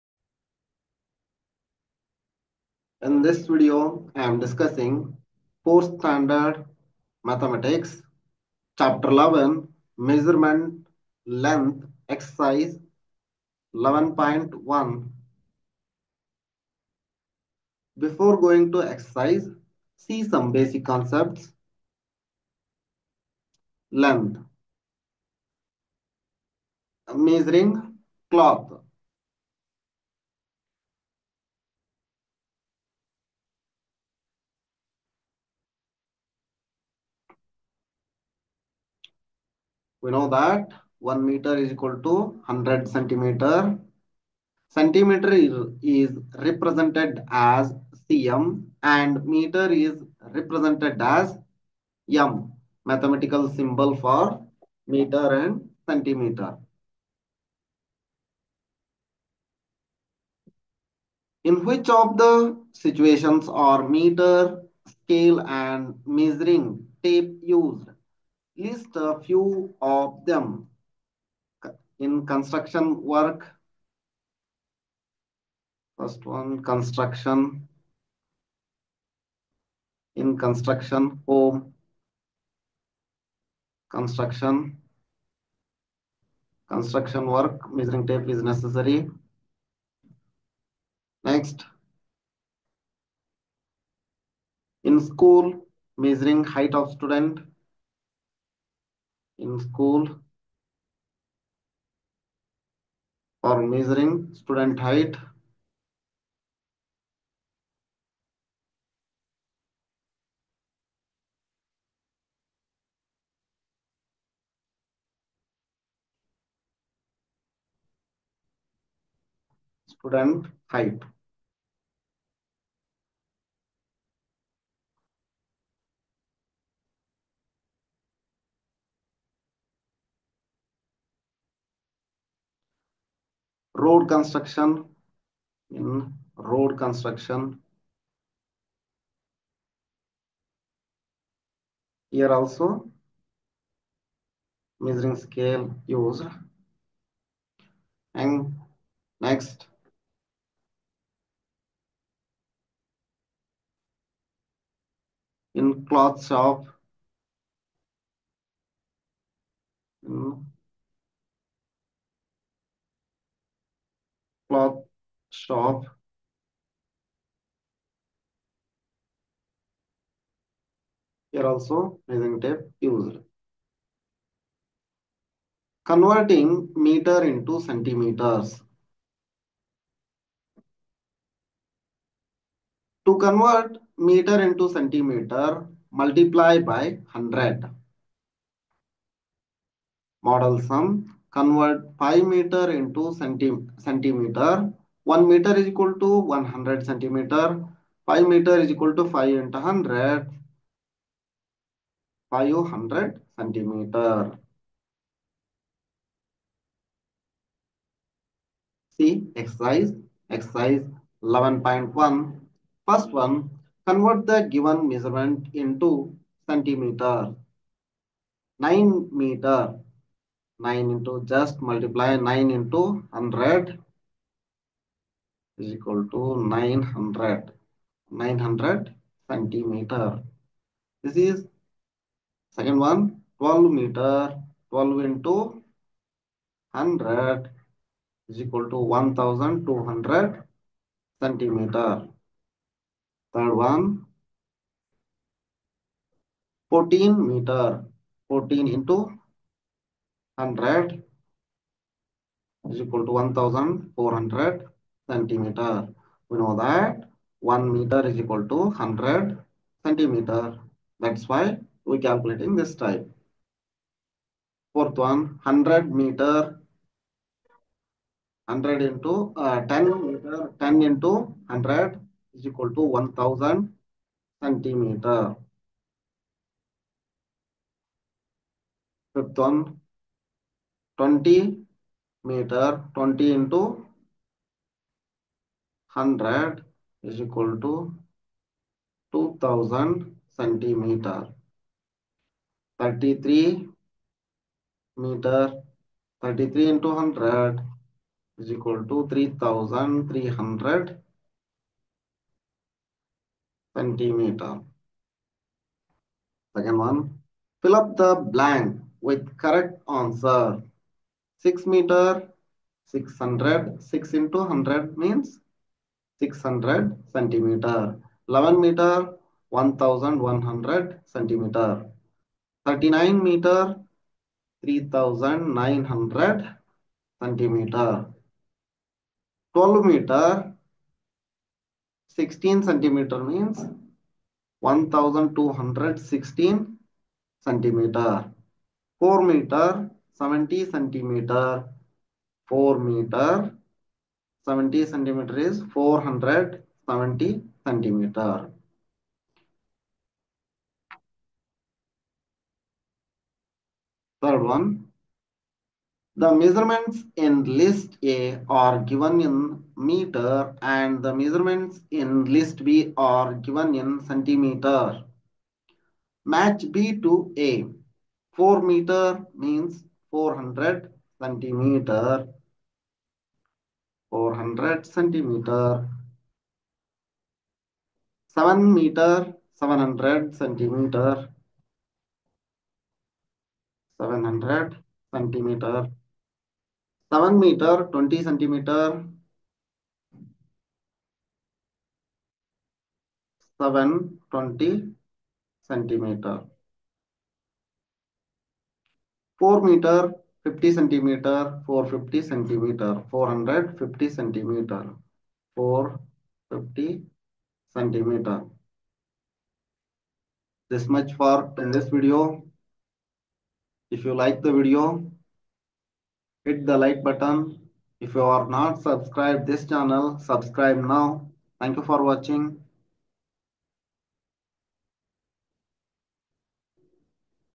4th std Mathematics | Chapter – 11 MEASUREMENT-LENGTH Exercise 11.1 | English Medium
Converting meters to centimeters is simple. You just need to multiply the number of meters by 100,
because there are 100 centimeters in one meter.

Here are a few practice problems for converting meters to centimeters:
Problem 1:
Convert 3 meters to centimeters.
Solution:
3 meters × 100 = 300 centimeters

Problem 2:
Convert 7.5 meters to centimeters.
Solution:
7.5 meters × 100 = 750 centimeters

Problem 3:
Convert 0.25 meters to centimeters.
Solution:
0.25 meters × 100 = 25 centimeters

Problem 4:
Convert 12.8 meters to centimeters.
Solution:
12.8 meters × 100 = 1280 centimeters